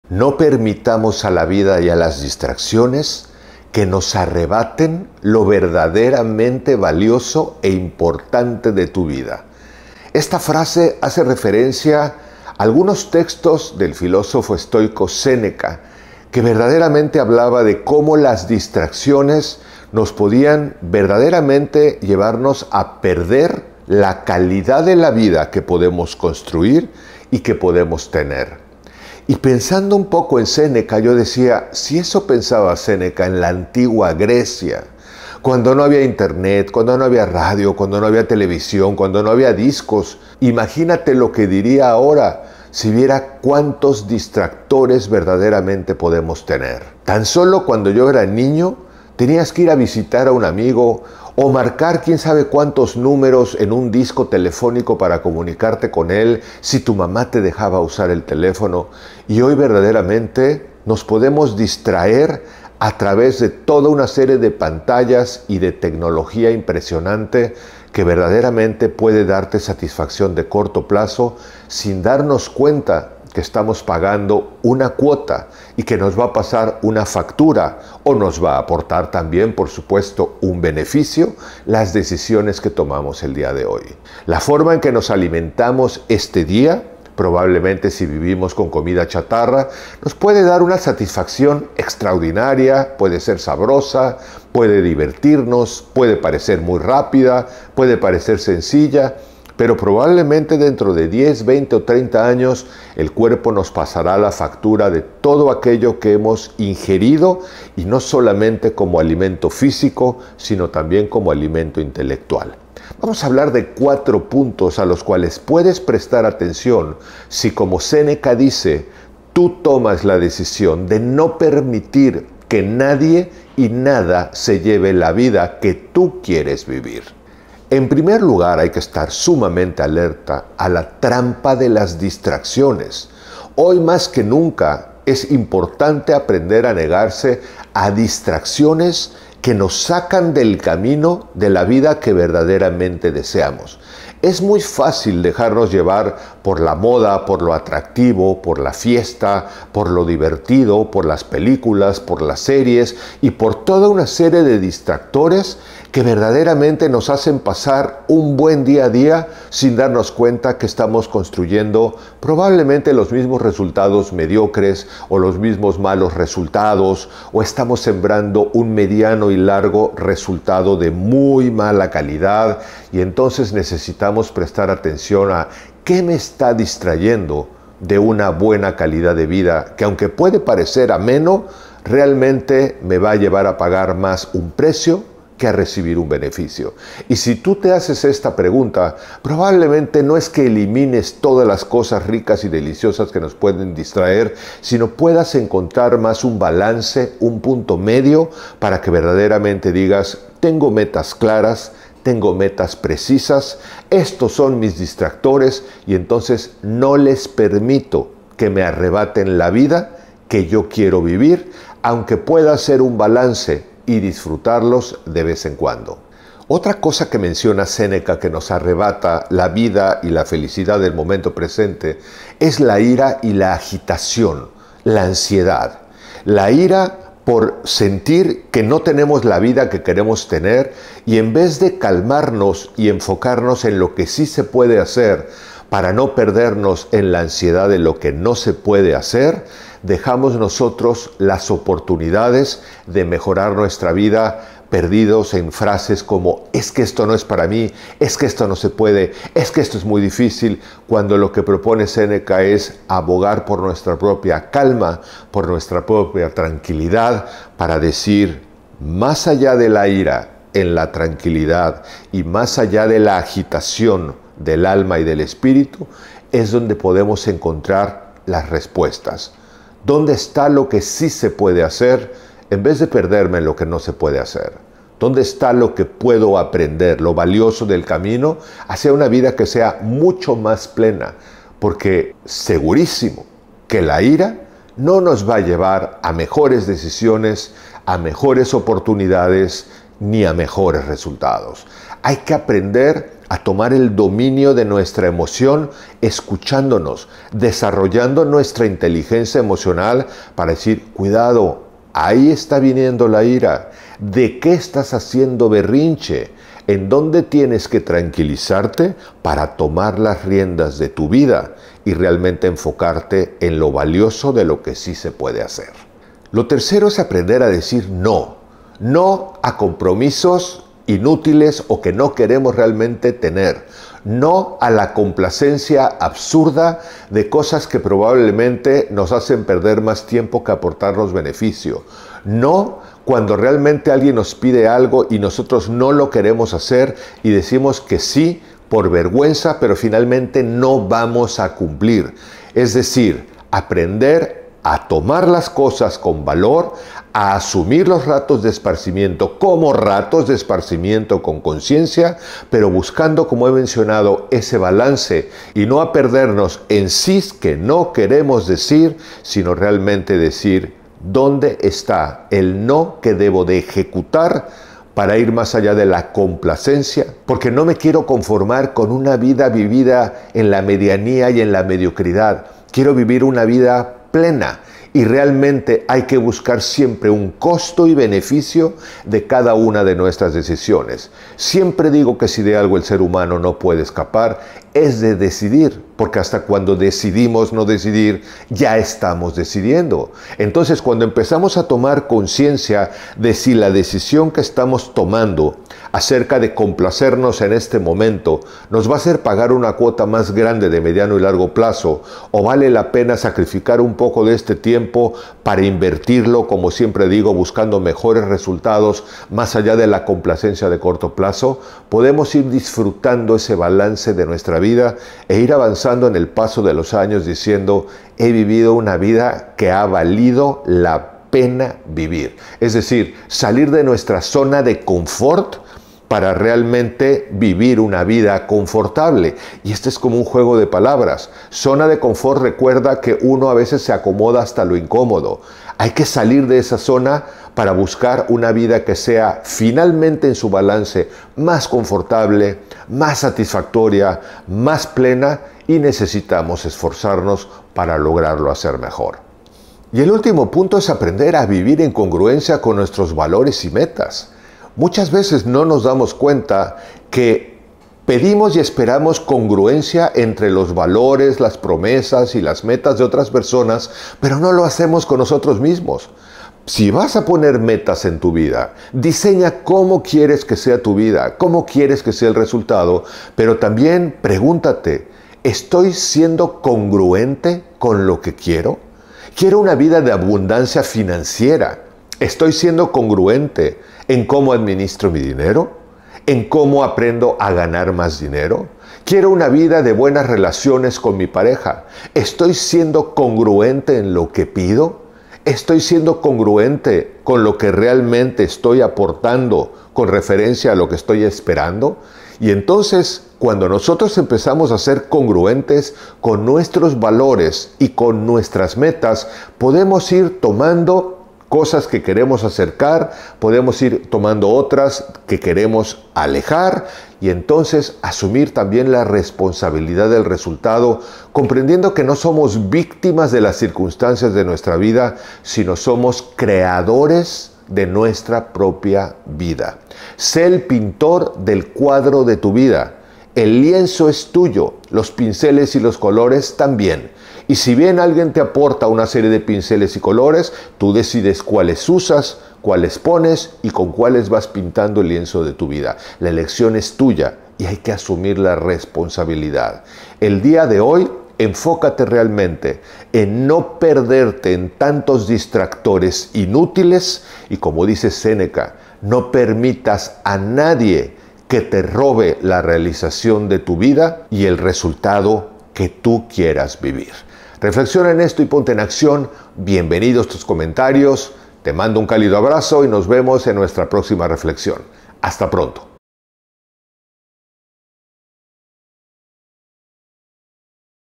No Permitas que te Roben lo que Realmente Importa
🔴 EN ESTE VIDEO: En nuestro afán por alcanzar una vida plena y significativa, a menudo nos enfrentamos a tentaciones y distracciones que nos apartan de lo verdaderamente importante. Siguiendo las palabras del filósofo estoico Séneca, es crucial que no permitamos que nos arrebaten la vida sin siquiera percatarnos de que la estamos perdiendo.

00:00 Séneca
02:07 La trampa de las distracciones
03:36 Renunciar a la ira y a la agitación
05:15 Decir "no" a la complacencia
07:29 Vivir en congruencia con nuestros valores y metas